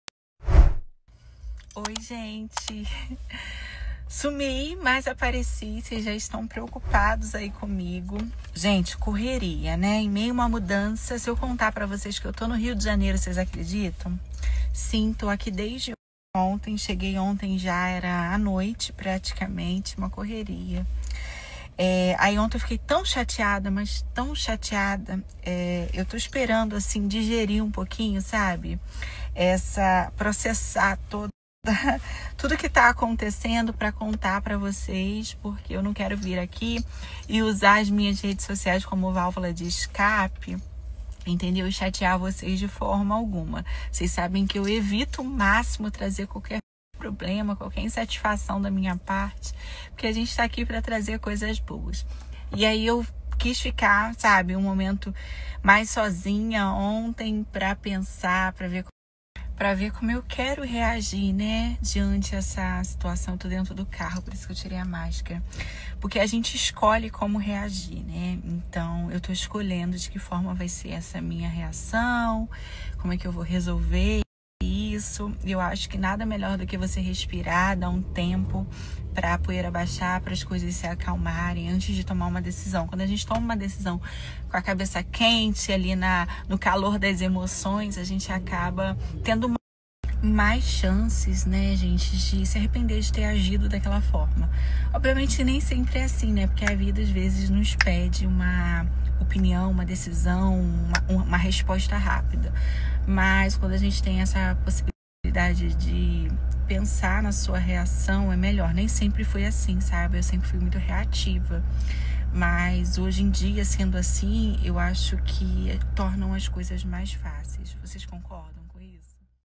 Mari Gil está passando por problemas e viaja sozinha para o Rio de Janeiro.
Olá Tutubes! Tudo bem com vocês?

A Youtuber Mari Gil deixou no ar que está passando por problemas, e resolveu viajar sozinha para o Rio de Janeiro para tomar decisões.

Confiram o vídeo.

Compartilhe